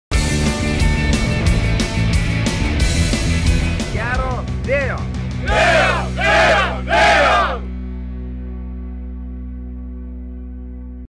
St. Maarten Heineken Regatta - Team Ambersail's wishes to Rokas
The St. Maarten Heineken Regatta takes place the first full weekend in March each year. for the latest information. Come join us for some Serious Fun!